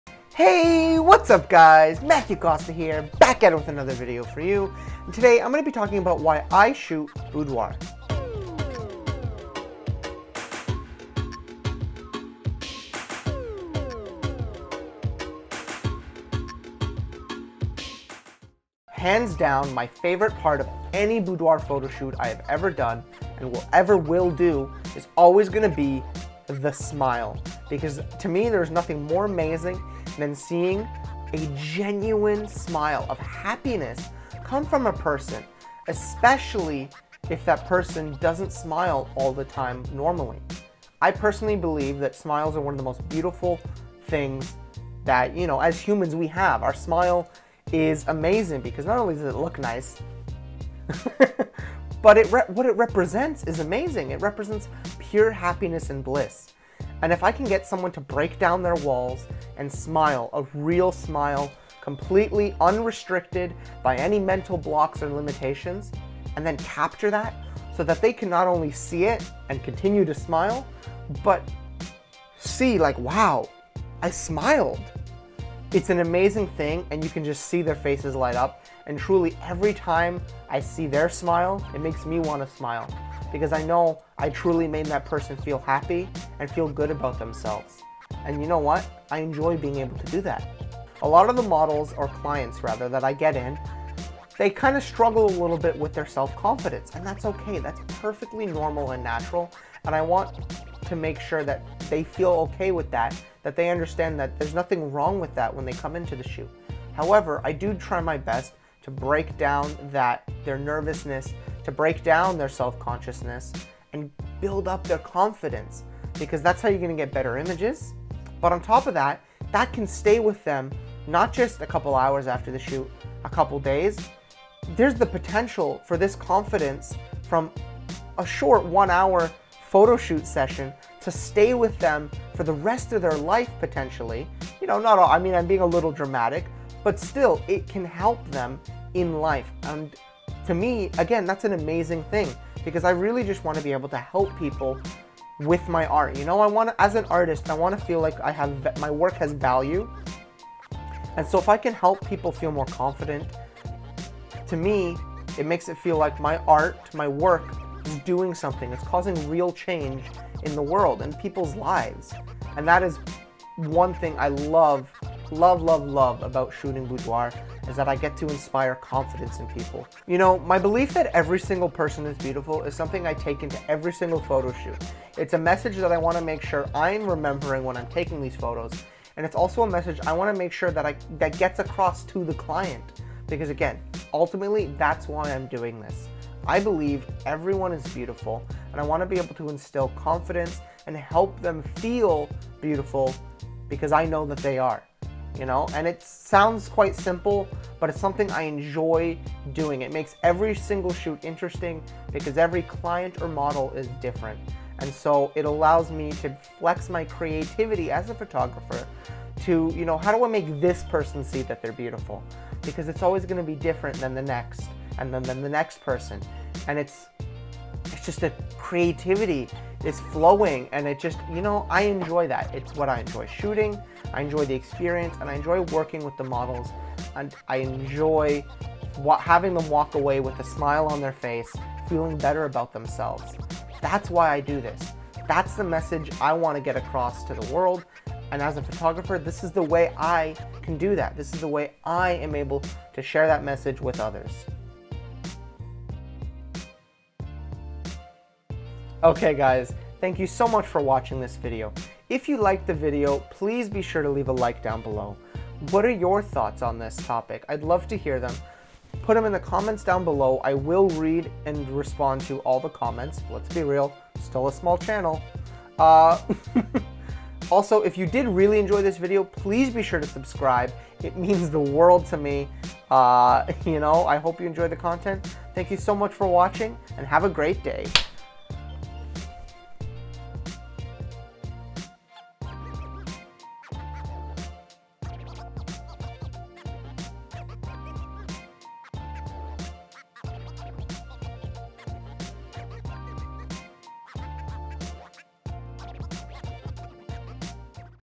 Why I shoot boudoir.
Hey guys! Today I am going to be telling you all about why shoot boudoir and what I love about it. By far my favourite part of any shoot is the pure and natural smiles i am able to get from my clients. Making them feel happy and confident. There is no better feeling for me than being able to make someone see themselves as beautiful.